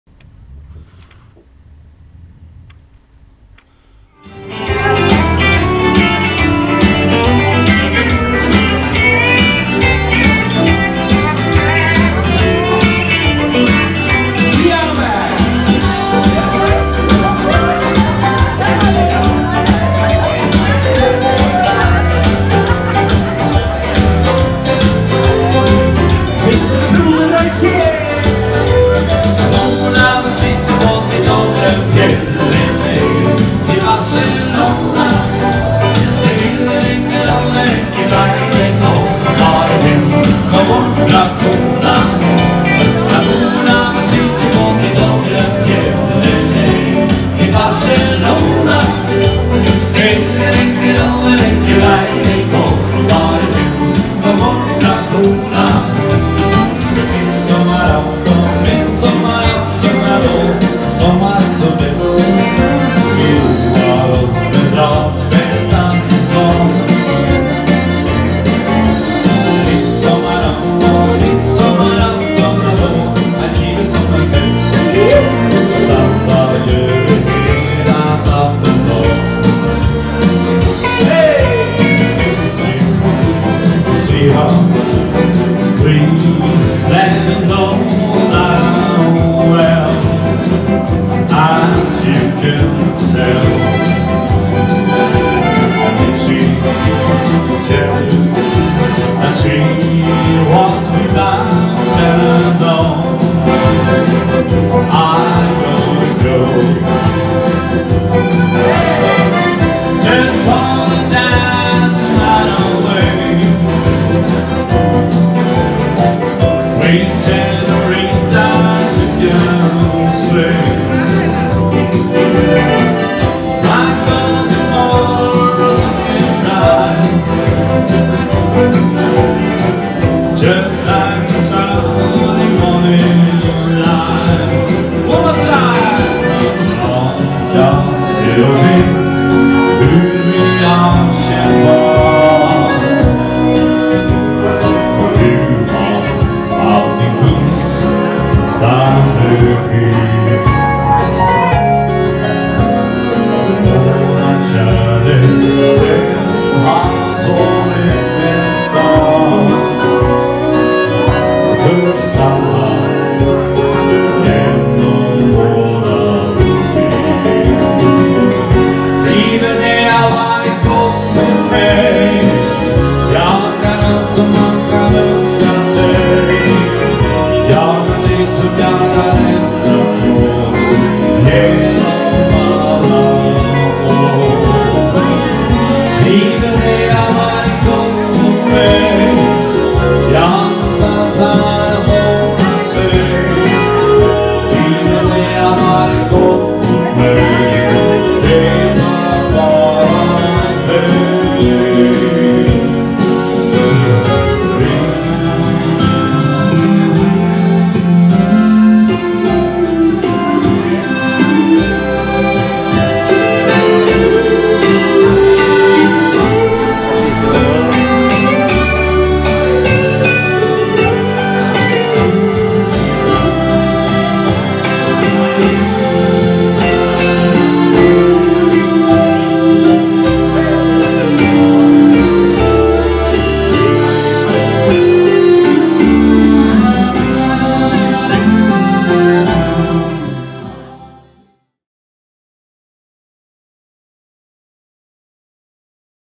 Tottis Bandet på Oset Høyfjellshotell 2019
TOTTIS på Oset Høyfjellshotell på Golsfjellet 6.-7. desember 2019.
Denne gangen med utvidet besetning.
Tottis kan bookes både som duo, trio og band.
Booking tlf nr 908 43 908. Se ellers facebook - Tottis Bandet